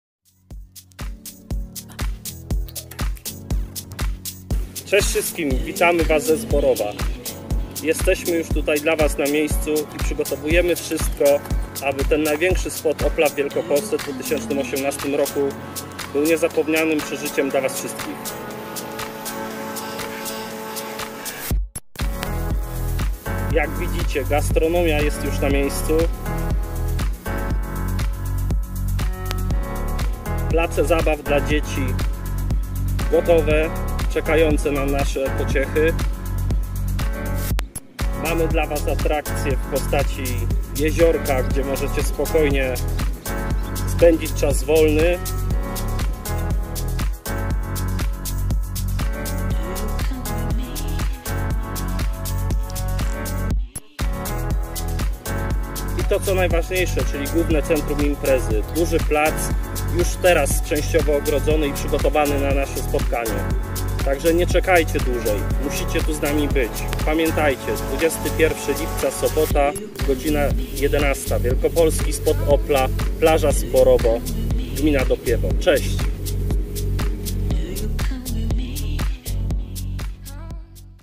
Wielkopolski Spot OPLA - 21.07.18 - Zwiastun
WSO - Wielkopolski Spot Opla - Plaża Zborowo nad jeziorem Niepruszewskim - sobota 21.07.18 - Start godzina 11:00 - ZAPRASZAMY :)